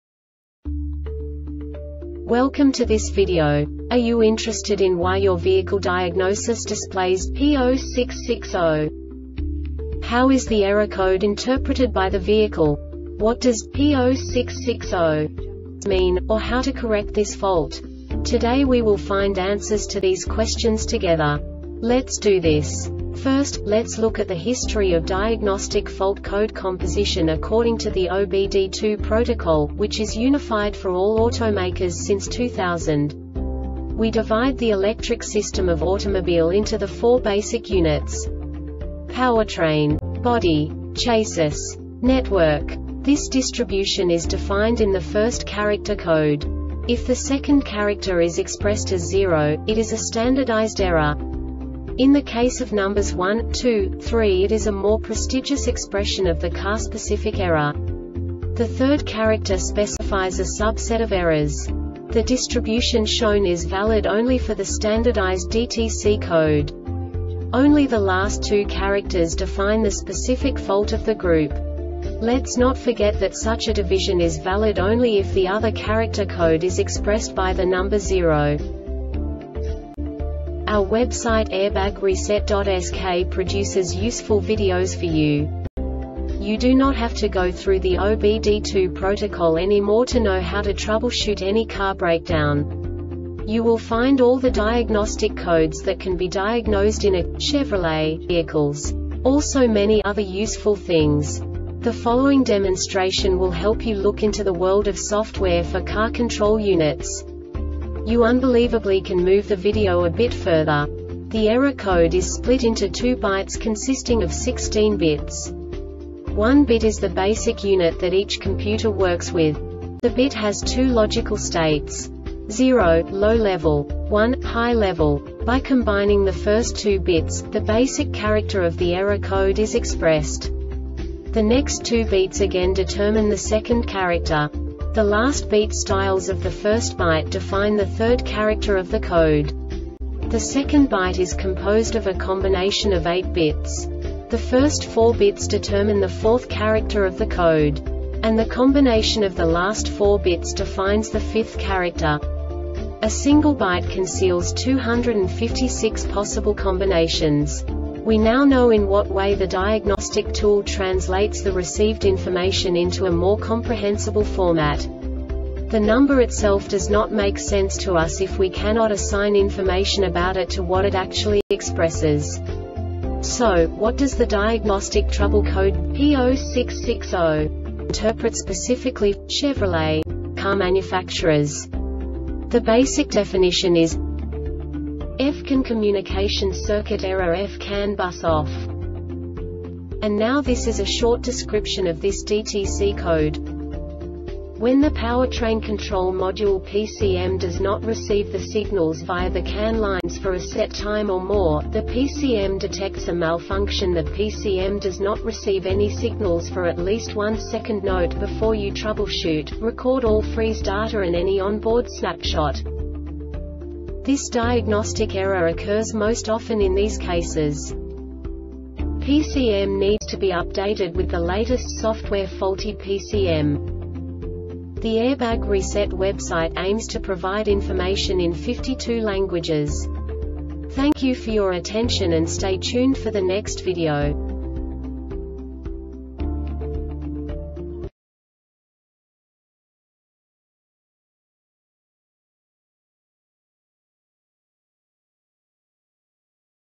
DTC Chevrolet P0660 Short Explanation
Contents:
0:21 Basic DTC analysis according to OBD2 protocol standard.
1:48 Insight into programming
2:58 A diagnostically understandable description of the Diagnostic Trouble Code
3:05 Basic error definition

Make:
Chevrolet
Code:
P0660
Definition:
F-CAN Communication Circuit Error (F-CAN Bus OFF)
Description:
When the powertrain control module (PCM) does not receive the signals via the CAN lines for a set time or more,the PCM detects a malfunction.The PCM does not receive any signals for at least 1 second.NOTE: Before you troubleshoot,record all freeze data and any on-board snapshot.
Cause:
 PCM needs to be updated with the latest software Faulty PCM